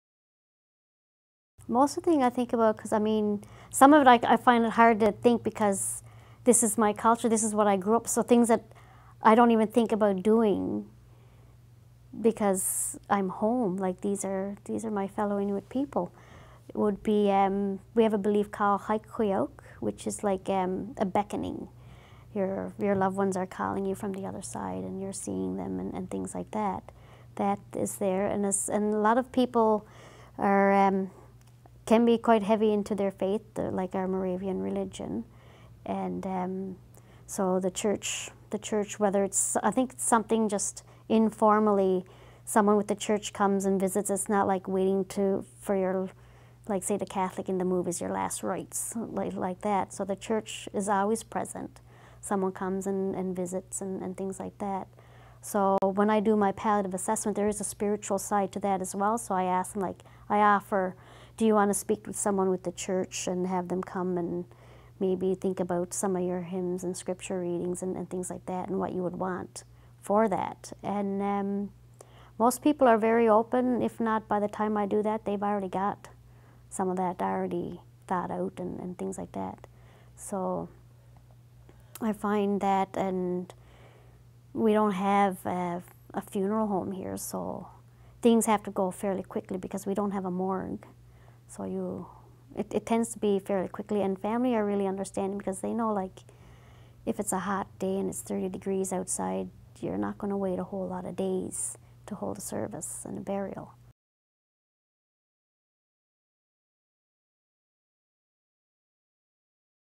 Sophie: Inuit perspectives on death and dying
In this video by Canadian Virtual Hospice, Sophie describes the Inuit understanding of death and after-death practices.